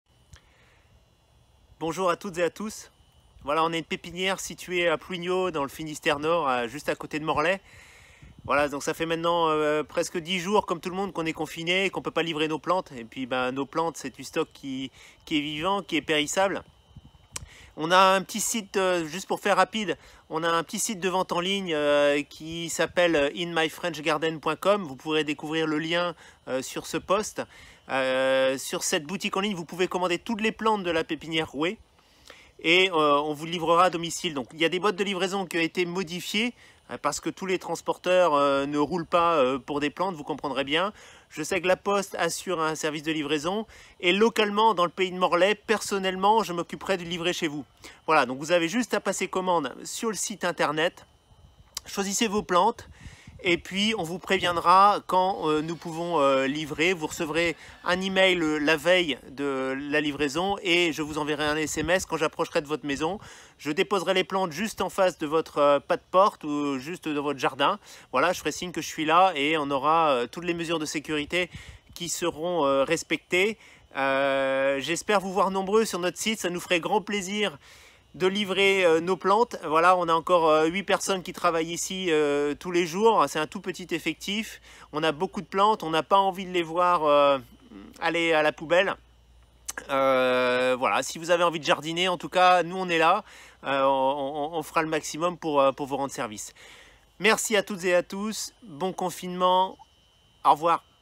Service de livraison exceptionnel de nos plantes à domicile sur toute la France !
Vous aimeriez jardiner et planter pendant cette période de confinement ? 👨‍🌾🌱👩‍🌾

In My French Garden a mis en place un service de livraison à domicile exceptionnel sur toute la France ! 🇫🇷

In My French Garden est un site de vente en ligne de plantes produites en Bretagne à Plouigneau 29610 dans le Finistère Nord. Toutes les plantes que nous vendons sont produites par les Pépinières Roué. 🌱

Deux solutions s’offrent à vous pour recevoir vos plantes chéries et continuer à assouvir votre passion, le jardinage, tout en évitant le contact avec d’autres personnes ! 📦

1) Livraison à votre domicile par le producteur Thomas Roué dans un rayon de 15km autour de Plouigneau (29610). Liste des villes concernées dans le lien ci-dessous

2) Livraison à votre domicile sur toute la France par transporteur (Colissimo ou Chronopost)

Par contre, nous vous conseillons de lire attentivement les 2 solutions ci-dessous et voir laquelle peut répondre à vos attentes

Merci infiniment à toutes les personnes qui commanderont nos plantes, nous espérons qu’elles vous aideront à vous occuper pendant le confinement. Vous sauvez également des plantes qui ne demandent qu’à être plantées en cette saison de printemps ! ❤️🙏
▷ Dans cette vidéo :
Merci de votre fidélité ! Kenavo !